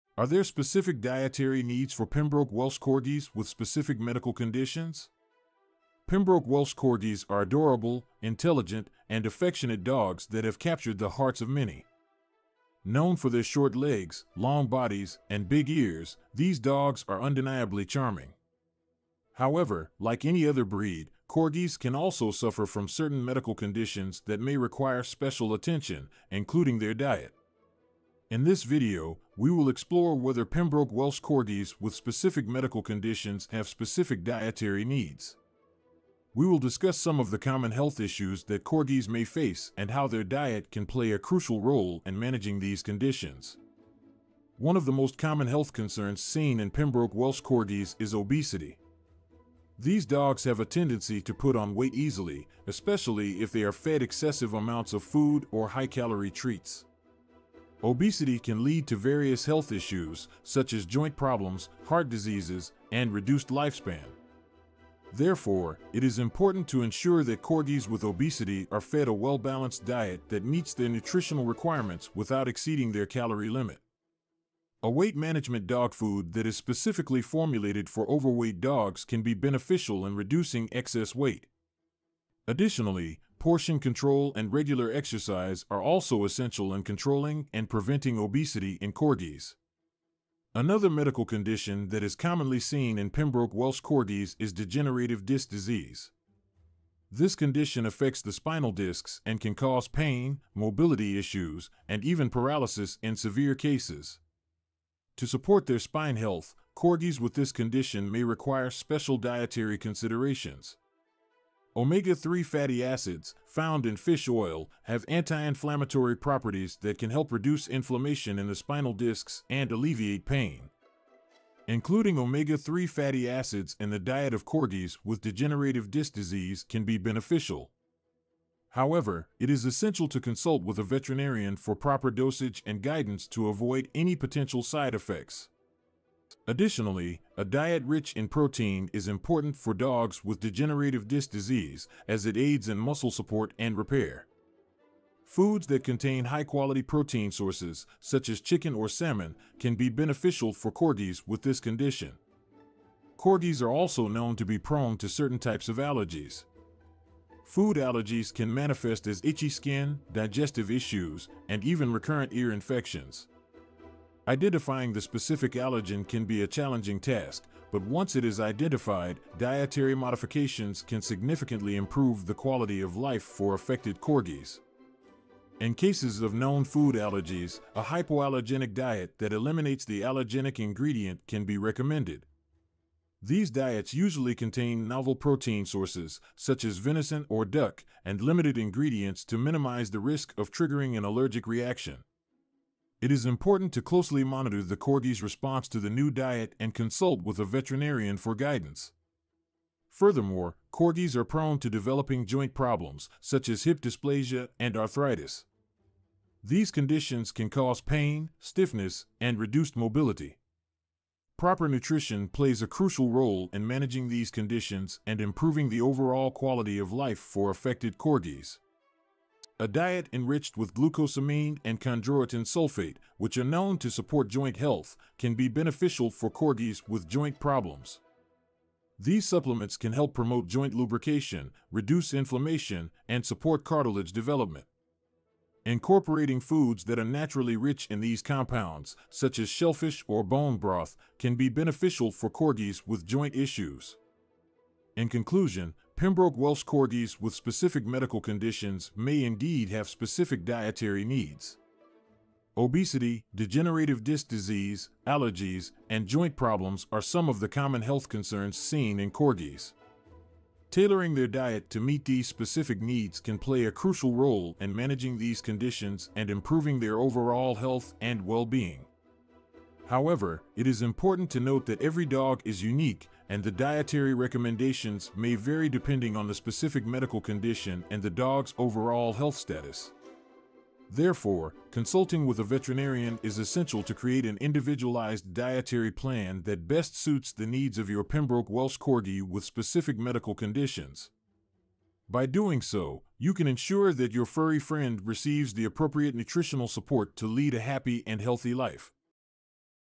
Special Dietary Needs for Pembroke Welsh Corgis with Medical Conditions
Pembroke Welsh Corgis are beloved companion dogs who require special attention when it comes to their dietary needs. This video discusses whether Corgis with specific medical conditions, such as obesity, diabetes, and joint problems, have specific dietary requirements. We will also explore the best food choices for these conditions. Keywords: Pembroke Welsh Corgis, dietary needs, medical conditions, obesity, diabetes, joint problems, best food choices.